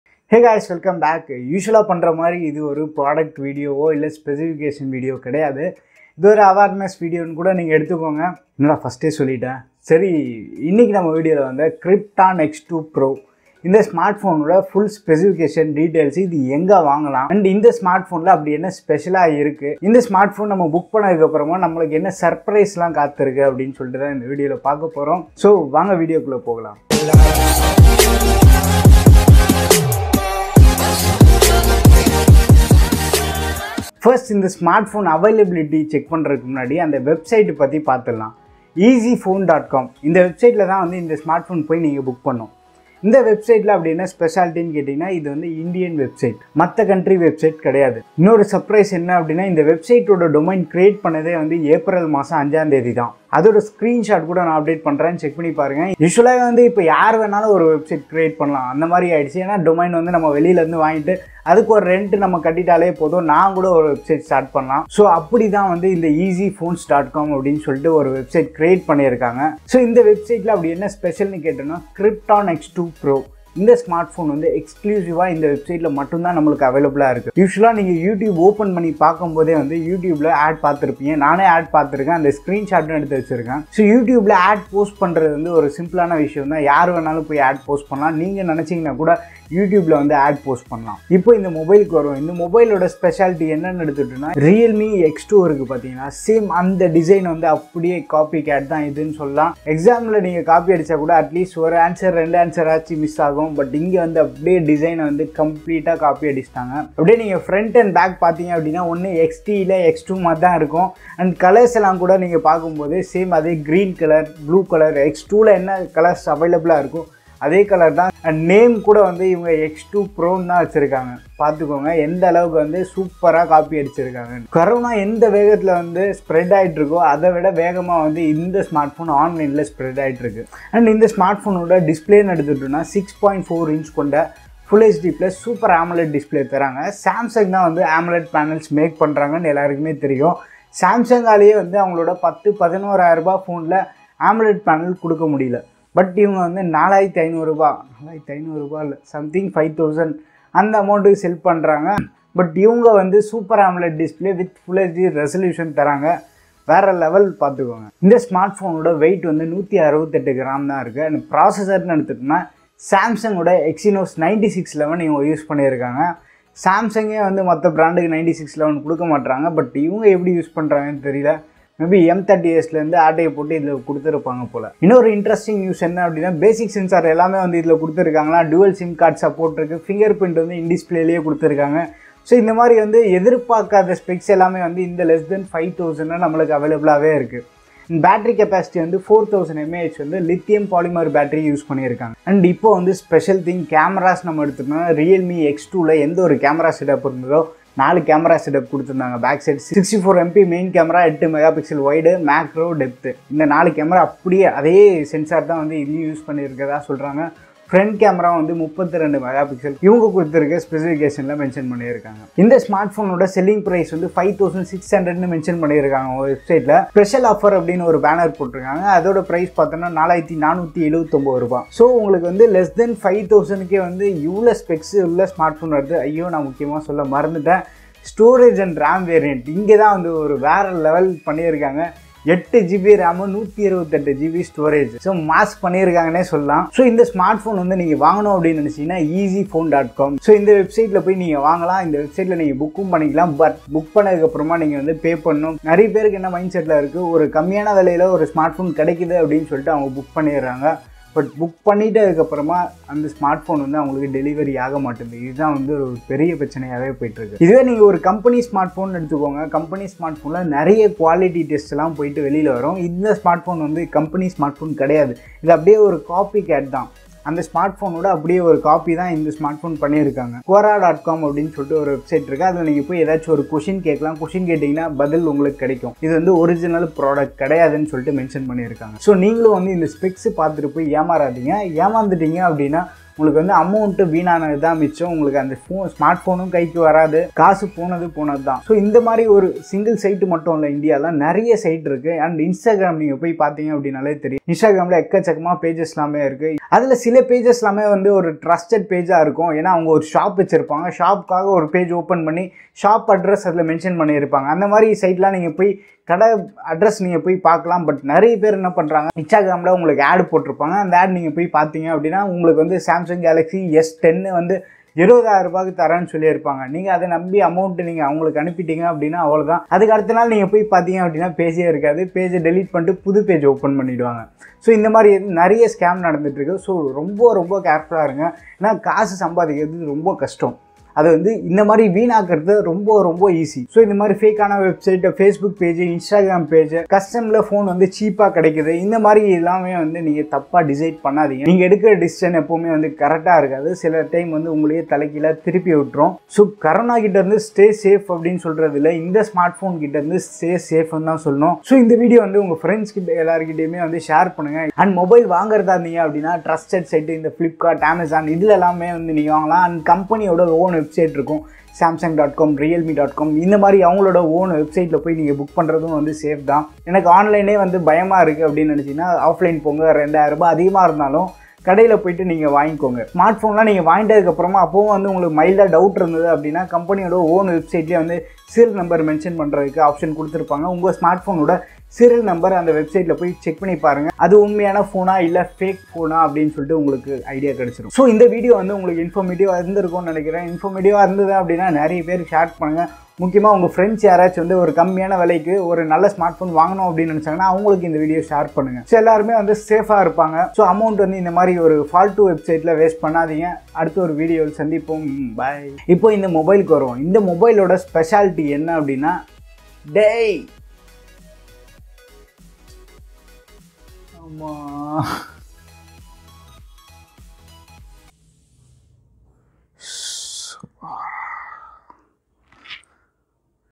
Krypton X2 pro full review in Tamil | yemaraadhinga be careful - 128gb (white) 8gb ram@4500
Krypton X2 pro full indpeth review

Hi friends , in this video we have discussed about the trending Krypton X2 pro mobile which is available in online at a dead cheap price.

website name -eezephone
so watch the video fiully and share your opinions in the comment section.
krypton x2 pro review in tamil
krypton x2 pro real or fake